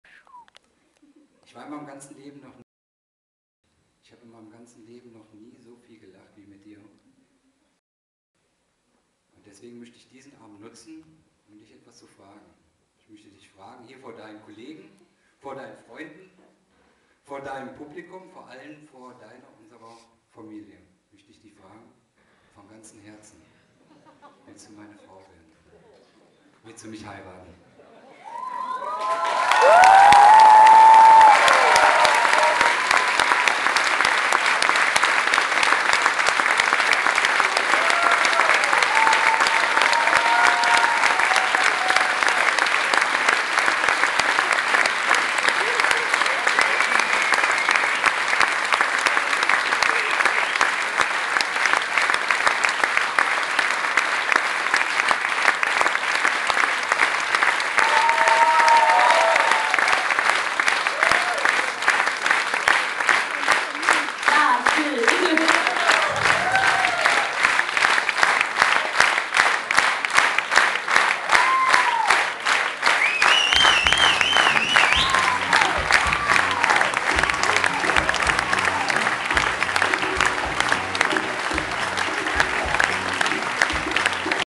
Musical Night
This was Friday, September 25th 2009 with Judith Lefeber and other german musical stars. And the boyfriend of Judith Lefeber asked her to marry him.